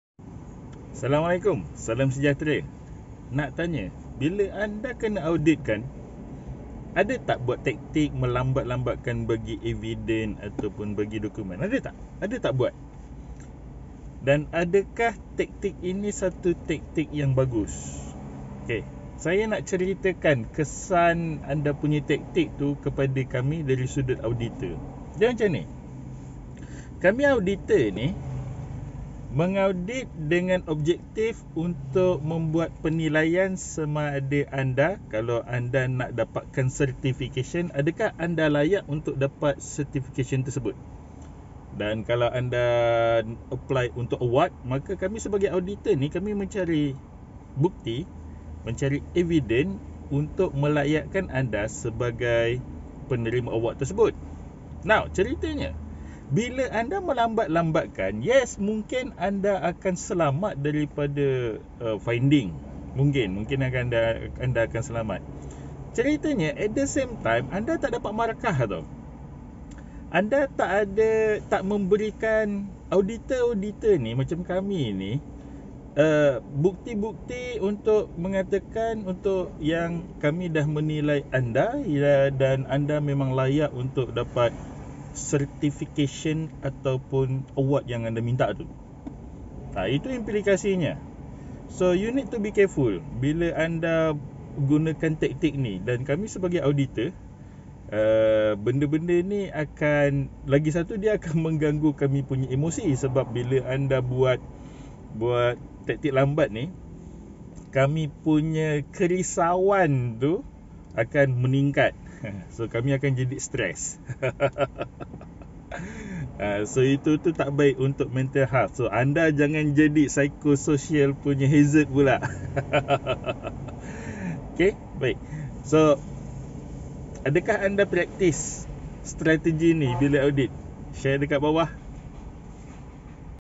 Adakah taktik melambatkan bagi dokumen atau evidence semasa audit itu bagus?
Pernah buat?

PM Coach bila anda sedang mencari in house trainer bulan ini